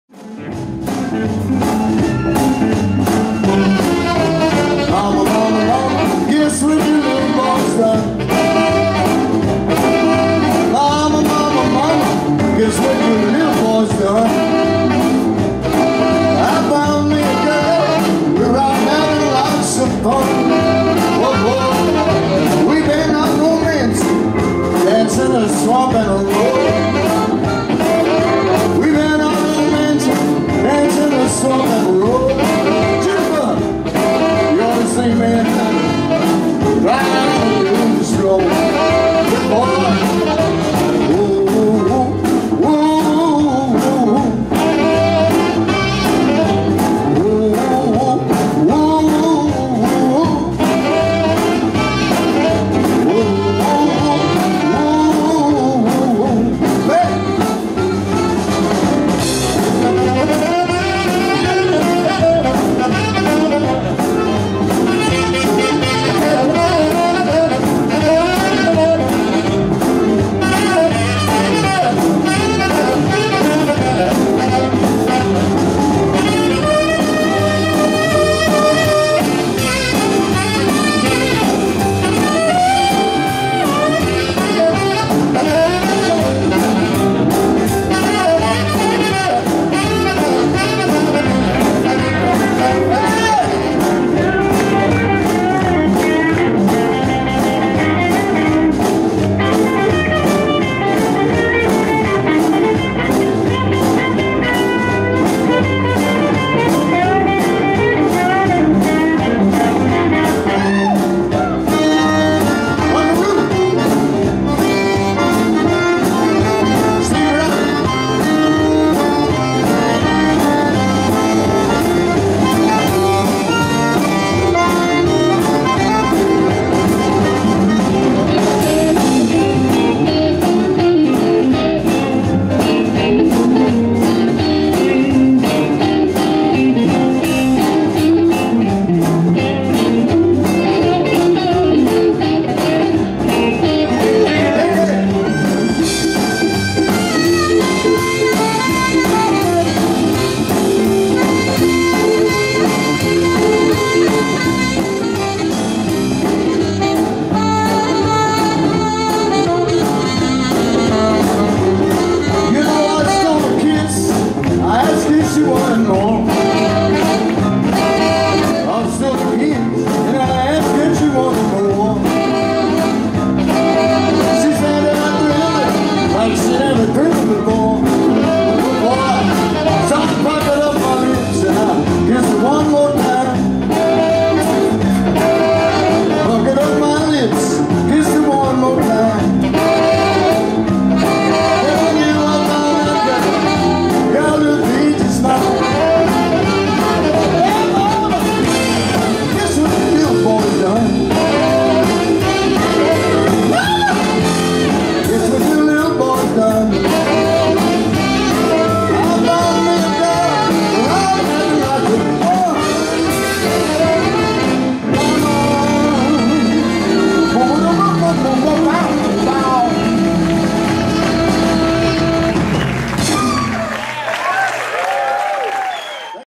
Lil' Band O' Gold in Lafayette, Louisiana
Lil' Band of Gold in Lafayette, La
Warren Storm, David Egan, Steve Riley, CC Adcock, David Ranson,Dickie Landry, Pat Breaux, John Troutman and Lil' Buck Senegal